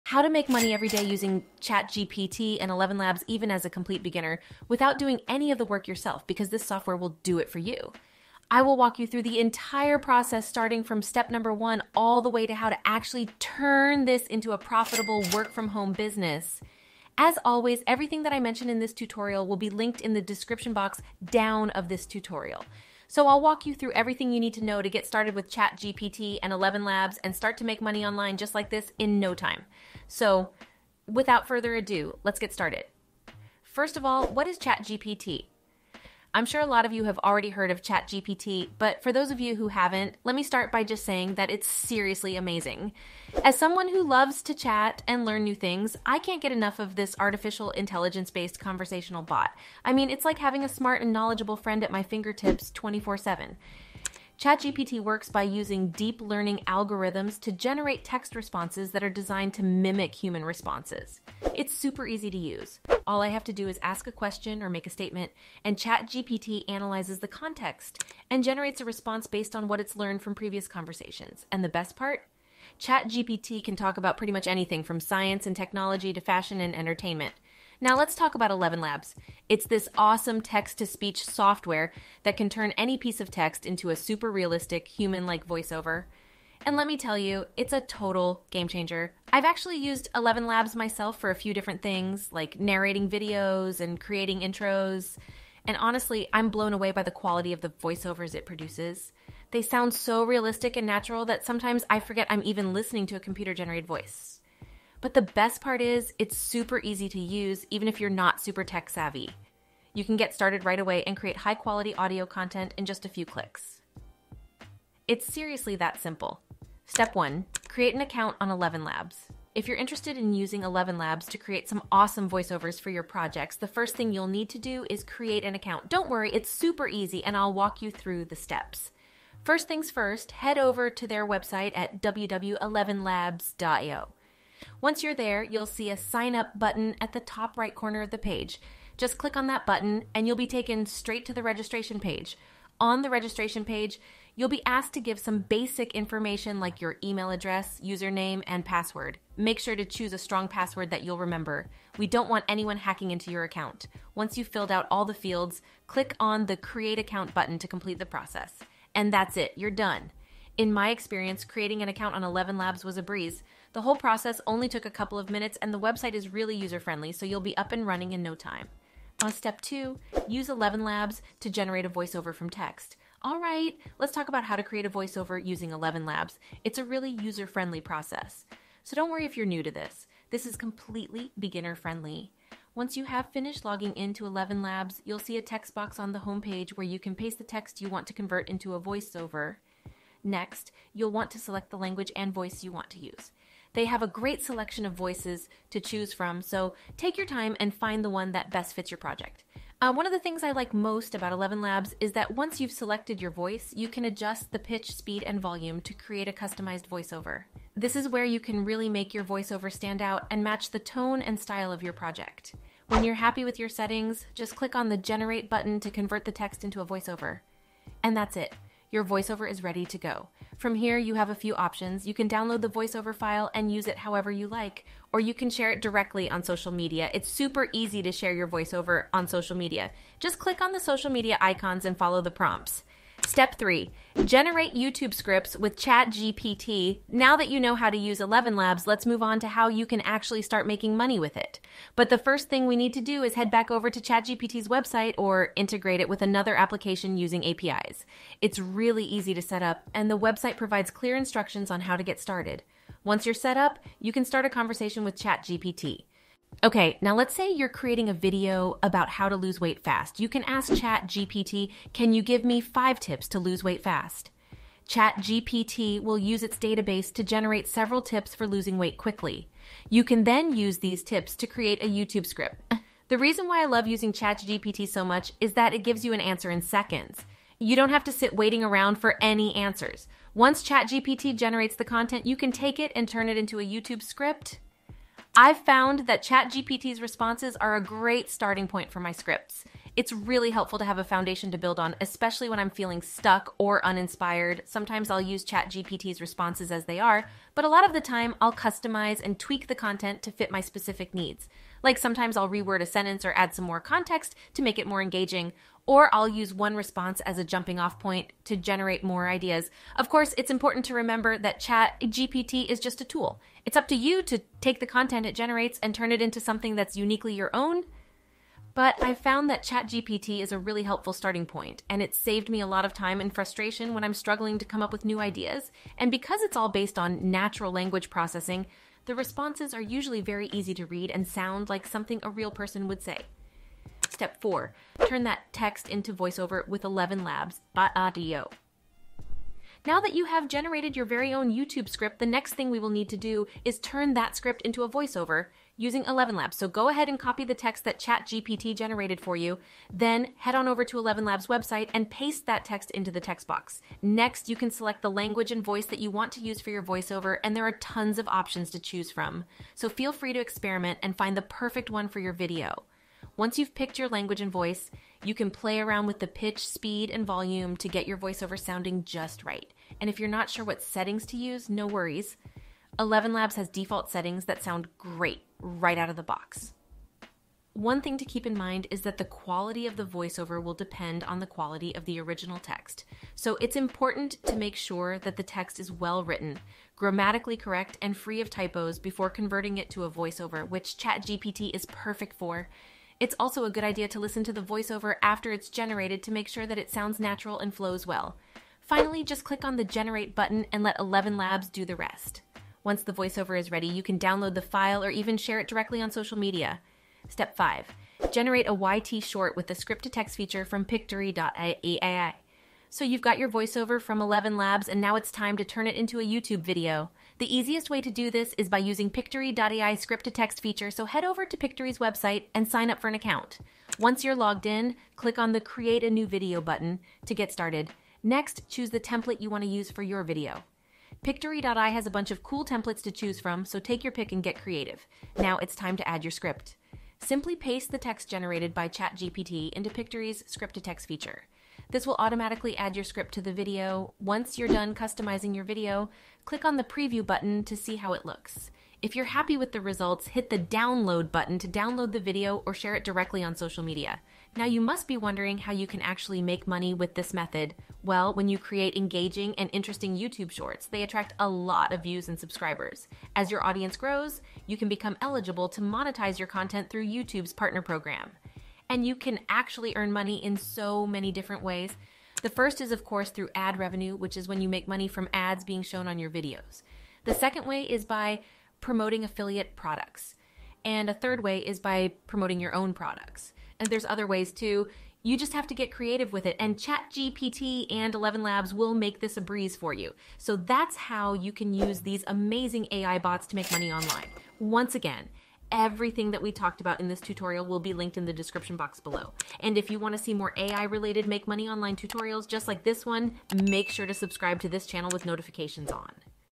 Best Way to Make Money Online with ChatGPT & ElevenLabs (Step by Step Tutorial)
In this step by step tutorial, we will be talking about how to make money online using ElevenLabs as a complete beginner worldwide!

ElevenLabs is the most realistic and versatile AI speech software, ever. Eleven brings the most compelling, rich and lifelike voices to creators and publishers seeking the ultimate tools for storytelling.